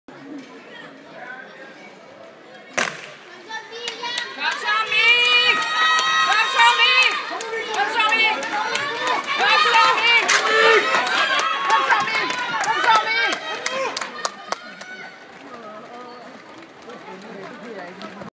Finale 60 m hæk Kronborg Indoor 2014
Magnus løber et flot finaleløb og får en 2. plads i finalen.
Vh. Peter